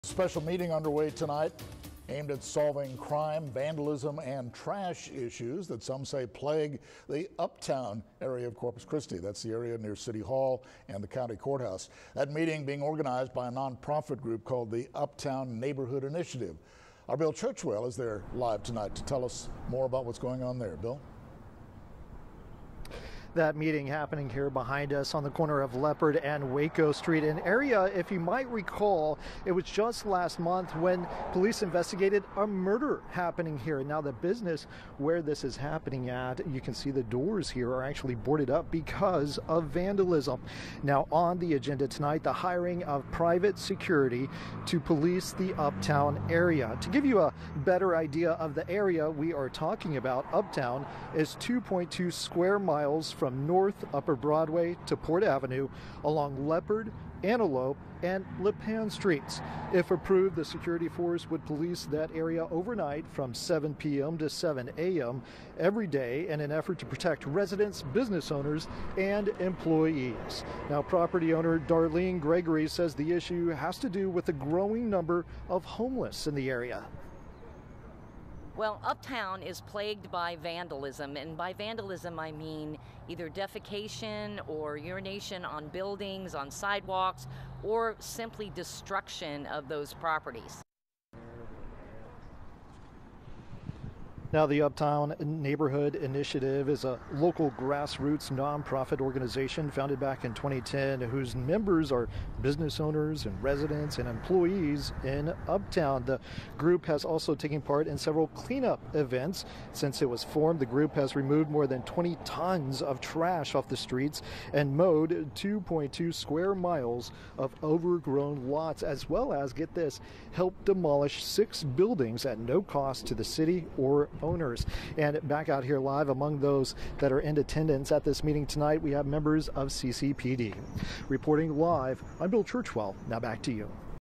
Special meeting to solve crime, vandalism, and trash in uptown Corpus Christi
To give you a better idea of the area, uptown is about 2.2 square miles from Upper North Broadway to Port Avenue along Leopard, Antelope and Lipan streets.